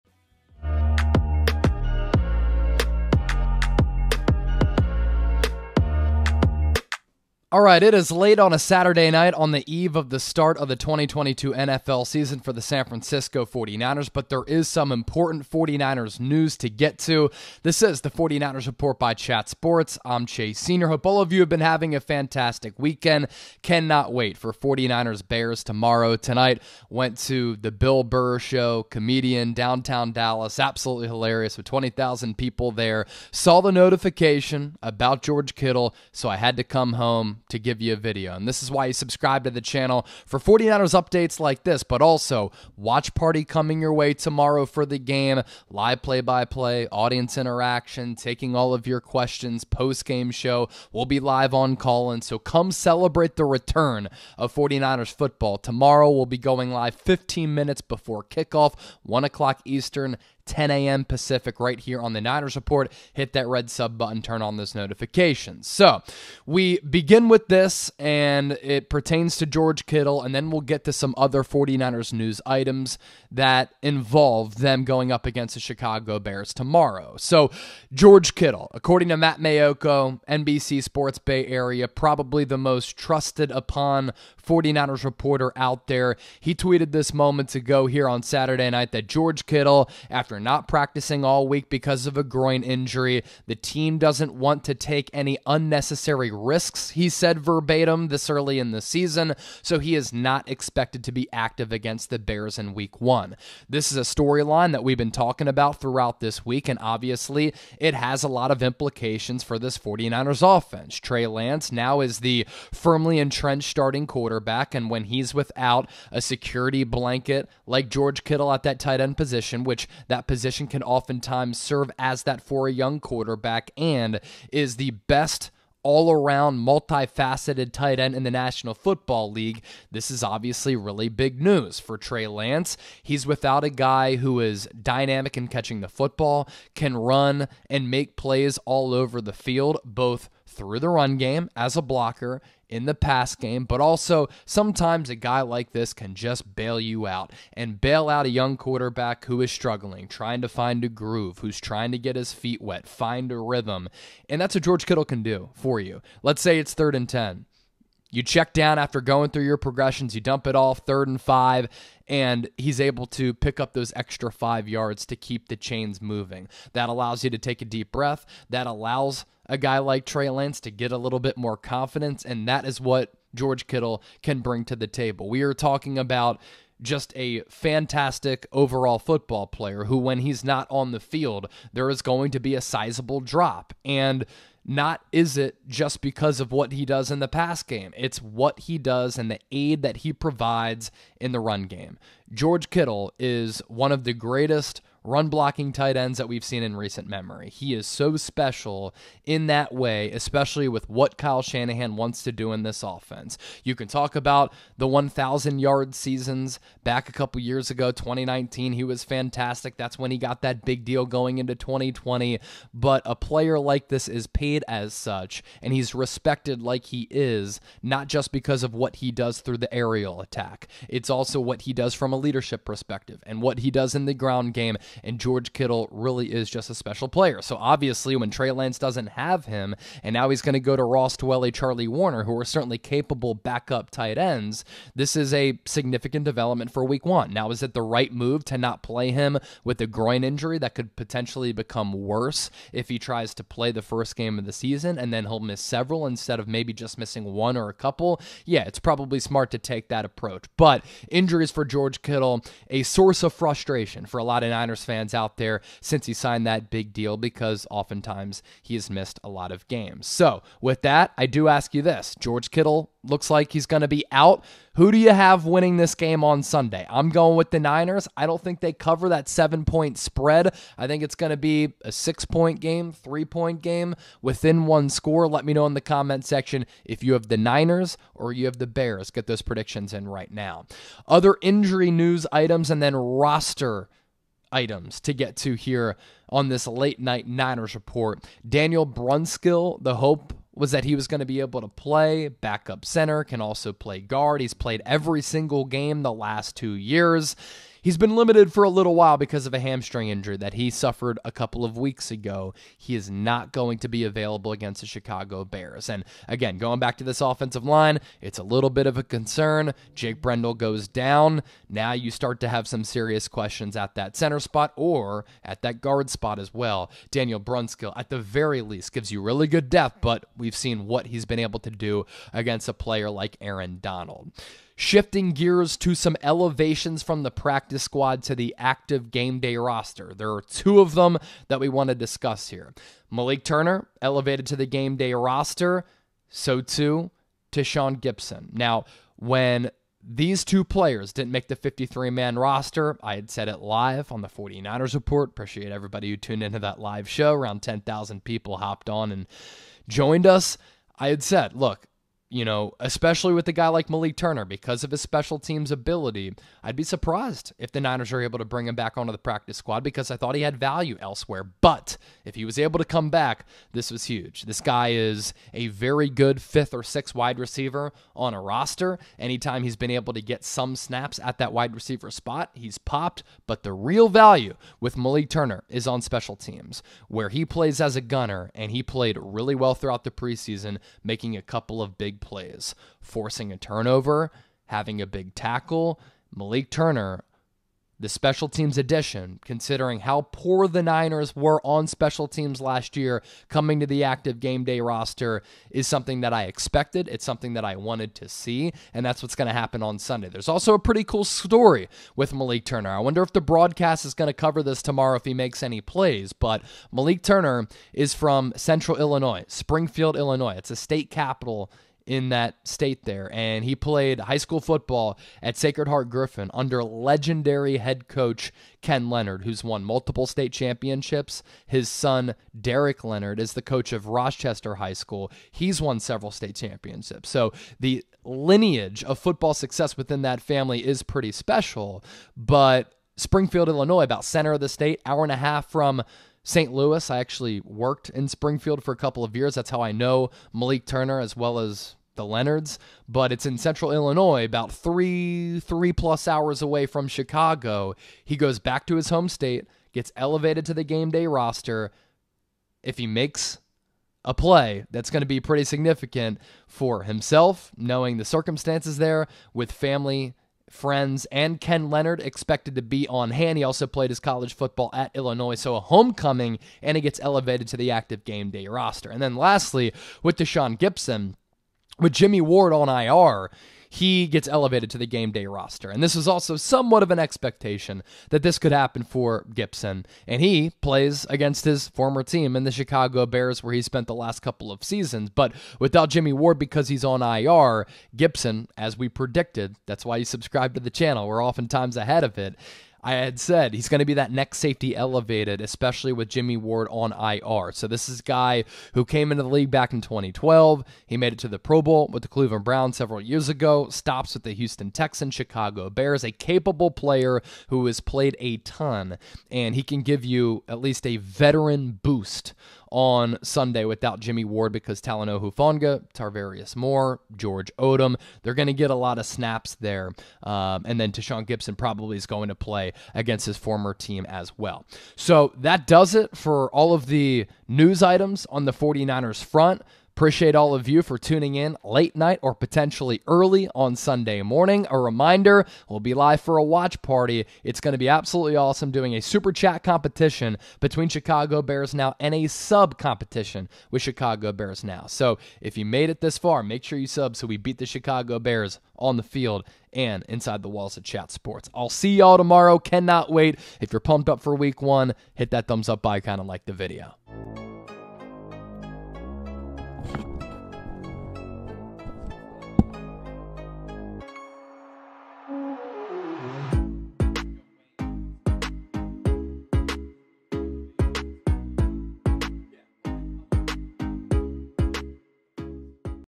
JUST IN: George Kittle NOT Expected To Play vs Bears | 49ers Injury Report, Malik Turner; 49ers News
We’re bringing you the latest 49ers news prior to NFL week 1 for 49ers vs. Bears and the latest on the 49ers injury report:
- George Kittle Injury News. According to Matt Maiocco of NBC Sports Bay Area, George Kittle is not expected to play vs. Bears because of a hamstring injury.
- Daniel Brunskill: Also not expected to play against Chicago because of a hamstring injury.
- Malik Turner has been elevated to the 49ers roster as well as Tashaun Gipson.

Come celebrate the return of 49ers football on the Niners Report

Hit up Chase Senior on social media!